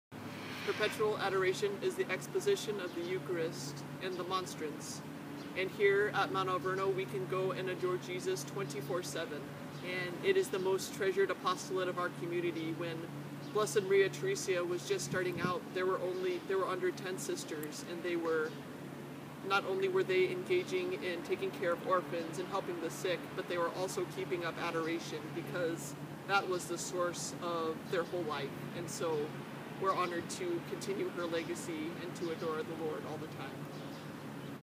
What is Perpetual Adoration?
Sr. Joan explains our charism of Perpetual Adoration.